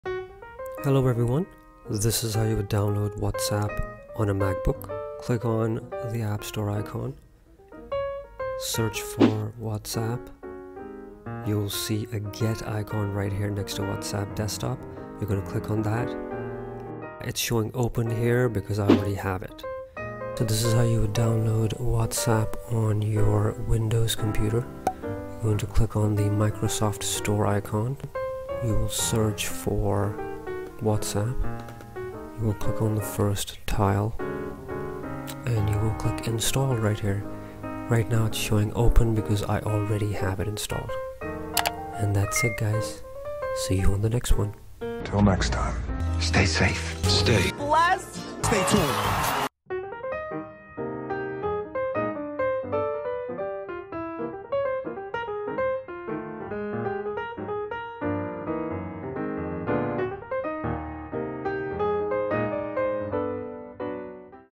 HOW TO Download WHATSAPP
How to download whatsapp? This is a short tutorial/training/overview video of how to download WhatsApp on Mac/Macbook and Windows.